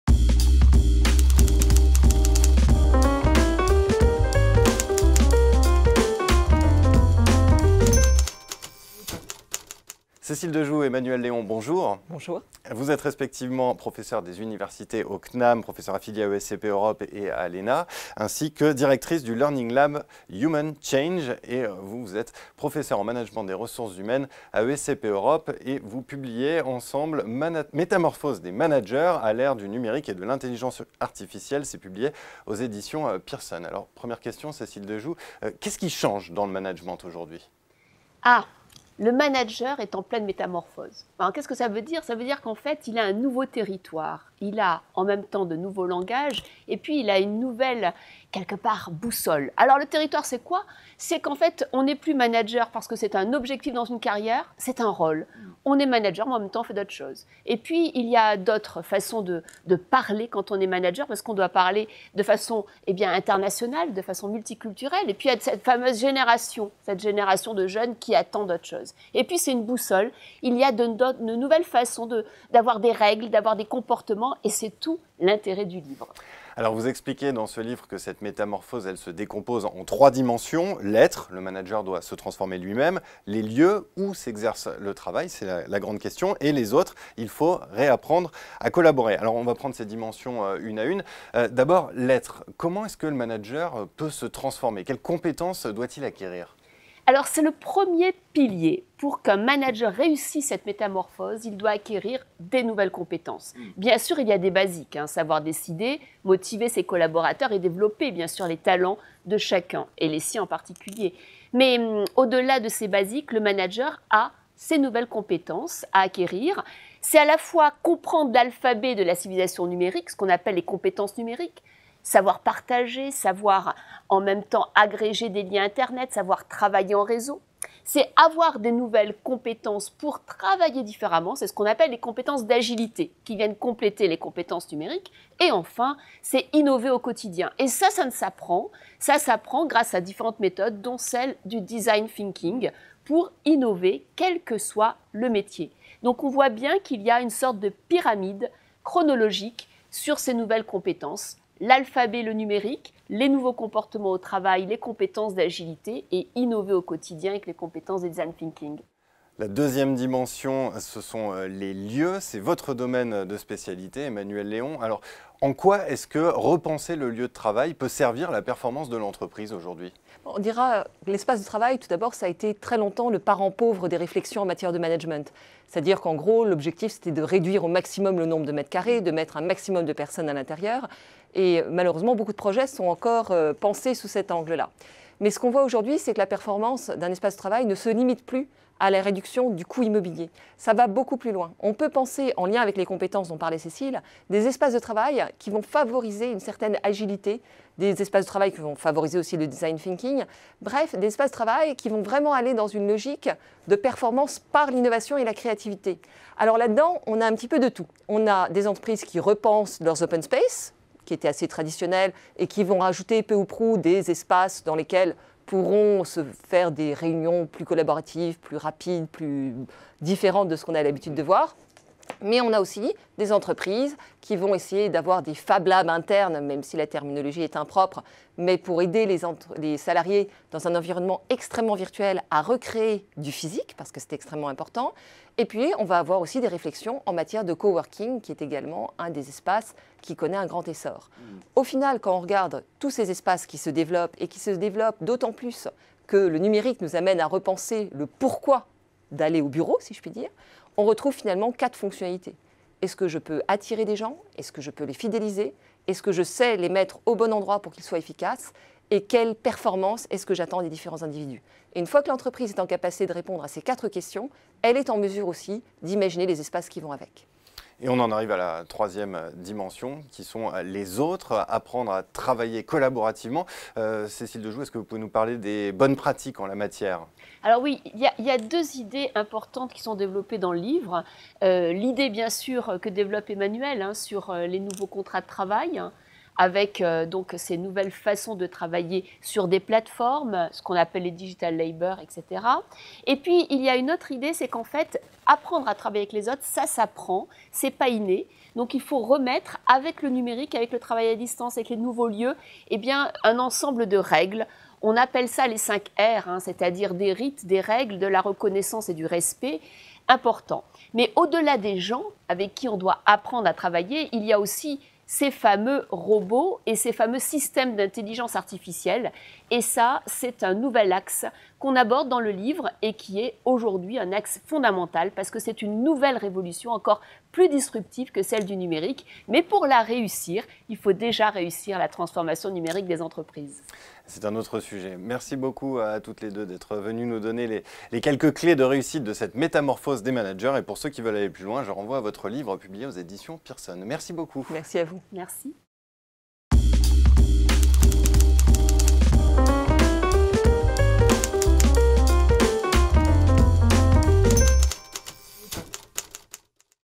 La métamorphose des managers face au numérique et à l'IA [Cécile Dejoux et Emmanuelle Léon]
Xerfi Canal a reçu Cécile Dejoux et Emmanuelle Léon, respectivement professeur des universités au conservatoire national des arts et métiers (Cnam) et professeur à l'ESCP Europe, dans le cadre de leur livre "Métamorphose des managers : à l'ère du numérique et de l'intelligence artificielle".